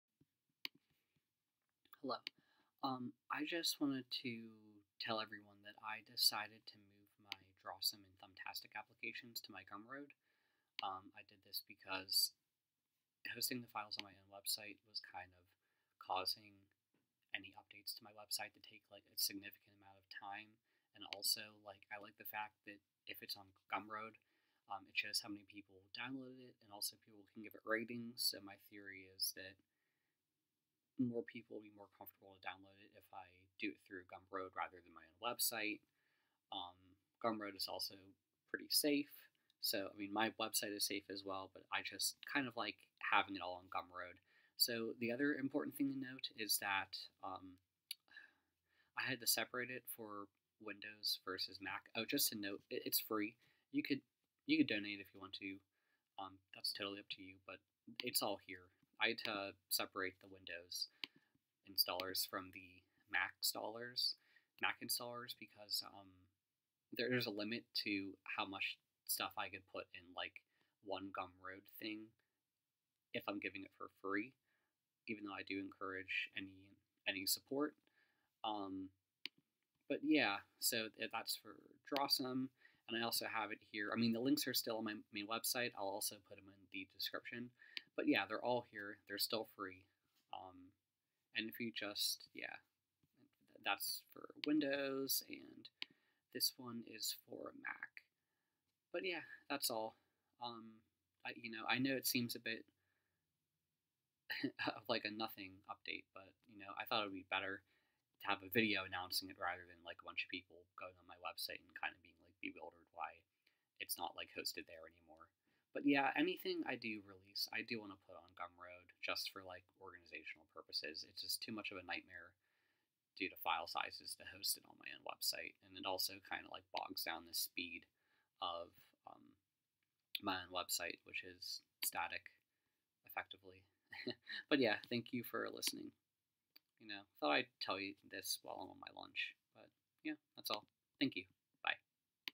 I moved Drawesome and Thumbtastic desktop apps to my Gumroad
This video is just an fyi that I moved my Drawesome and Thumbtastic desktop apps from my to my Gumroad late last night. At this point, I am exhausted as I was up late moving everything over trying to make it look perfect. The links on my website to my Gumroad exists. They can also be found here

The apps are both free and that is the way I plan to keep it. Any support is appreciated, but only if you can support yourself first.

An easier way to reference images: Thumbtastic 🖼️ (to easily reference photos)

Have a Question 🤔 about the video? Something didn't click? Perhaps I didn't explain something clearly.